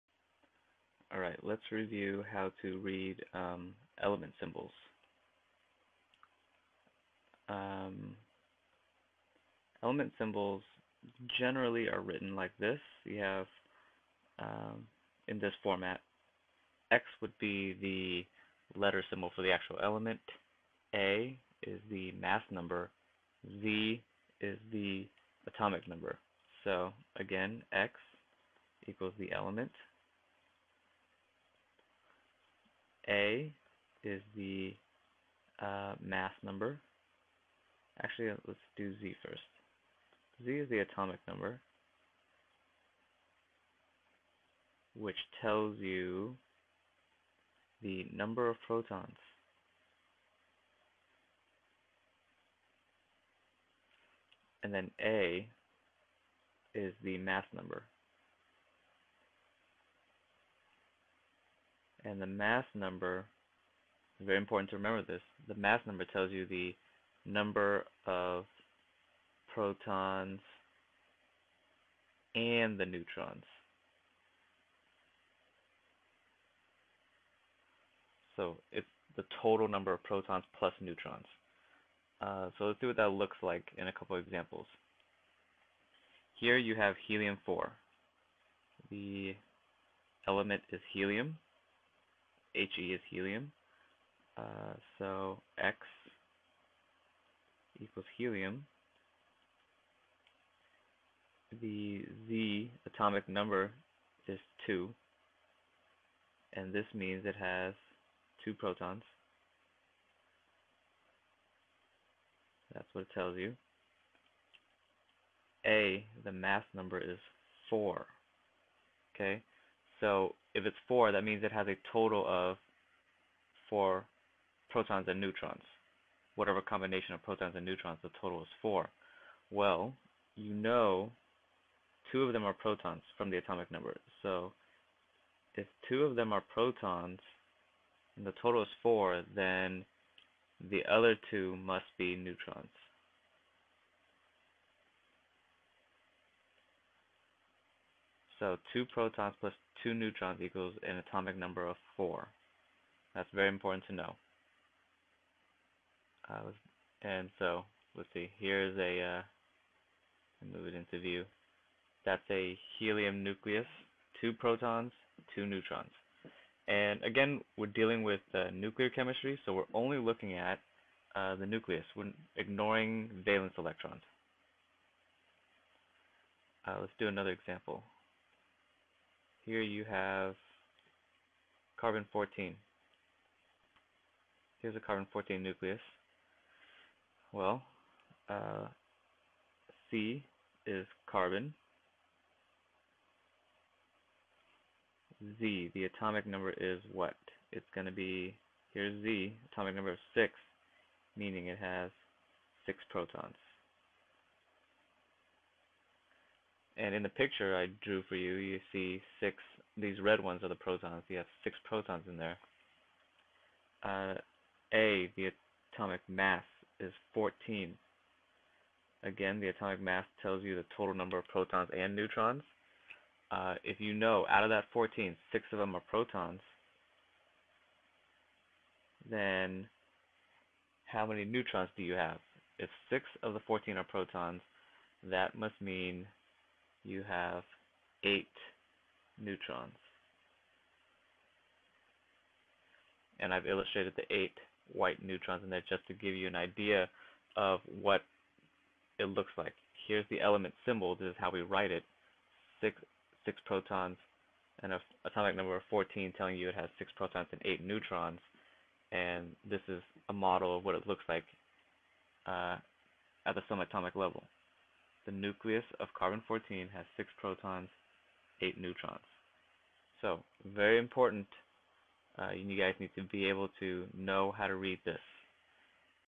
Reading Element Symbols
How to read element symbols. What the atomic number and mass numbers tell you.